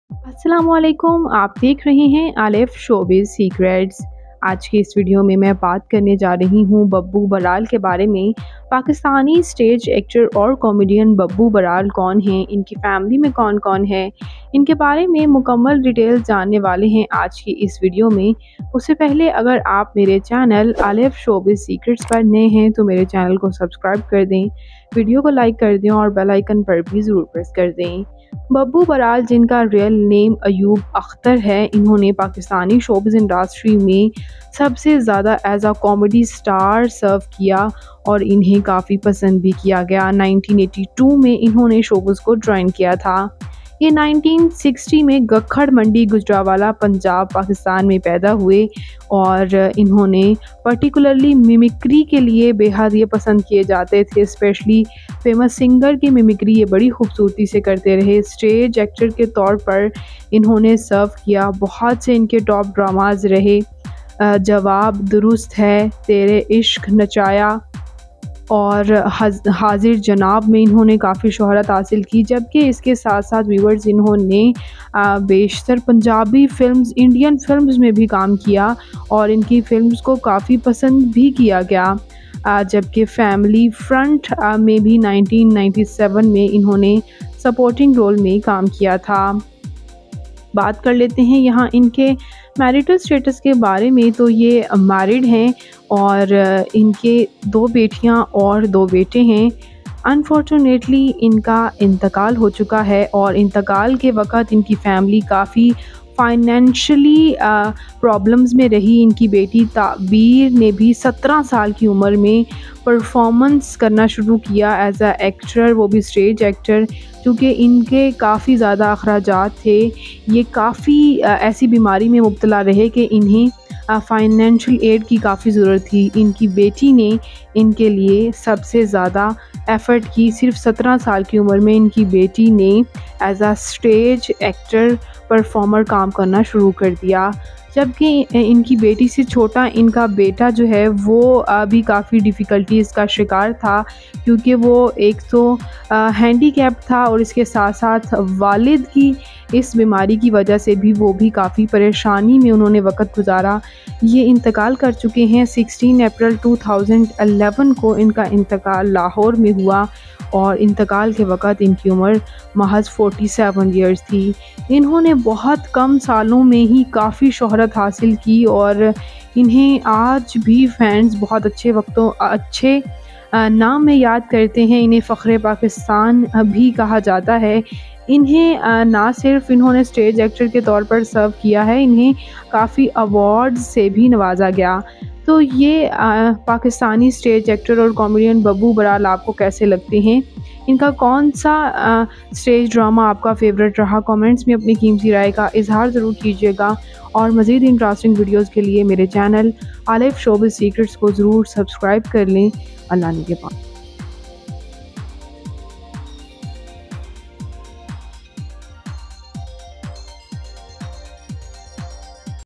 Babu Baral Untold Life Story - Babu Baral Family Biography 2024 - Alif Showbiz Secrets
Ayub Akhtar (ایوب اختر), better known by his stage name Babu Baral (ببو برال), was a Pakistani stage actor, comedian, singer, theatre director and playwright.

He was particularly known for his mimicry, especially of famous singers.[1]

Personal life
Babu Baral was born Ayub Akhtar in Ghakar Mandi, Punjab, Pakistan during 1964.[1]

He had four children, two daughters and two sons.[2][3]

After Baral's death his family struggled financially, pushing his elder daughter Tabeer to take up stage performances at the age of 17. His eldest son is handicapped whereas his second son has cerebral difficulties [4]

Career
Stage actor
Babu was originally a resident of Ghakar Mandi in Gujranwala who ventured into theatre back in 1982. After working in his hometown for some time, Babu moved to Lahore and soon took over the city's theatre circuit, thanks to his talent and hard work.

Over the next 20 years, the artist worked in over a thousand plays, with Shartiya Mithay and Bhukay Haatha Bataira being some of his biggest hits. In fact, it was not just Lahore that Babu had taken over; he went on to perform around the world and had become an asset for Pakistani theater during the peak of his career [5]

Theatre director and playwright
He not only acted but also wrote and directed plays, most famously Shartiya Mithay.[6]

Stage dramas
Shartiya Mithay
Topi Drama
Ashiqo Gham Na Karo
Suhay Lal
Kuch Na Kaho
Tere Nakhre Hazar
Mr. Aflatoon
Raja Ab To Aaja
Ghar Ayi Bharjayi
Baba Luteya Geya
Akhiyon Se Goli Maare
Balam bara Zalim
Buhat Khub
Mehndi Lagi Mere Hath
Siyane Bewaqoof
Hazir Janaab
Deewanay Mastanay
Jawab Durust Hai
Tere Ishq Nachaya
Mahi Mainu Chala Puwa Dey
Yaran Naal Baharan
Death
Babu Baral died on April 16, 2011. Babu had been suffering from various health issues, including cancer and kidney problems, for a long time before his death.[7]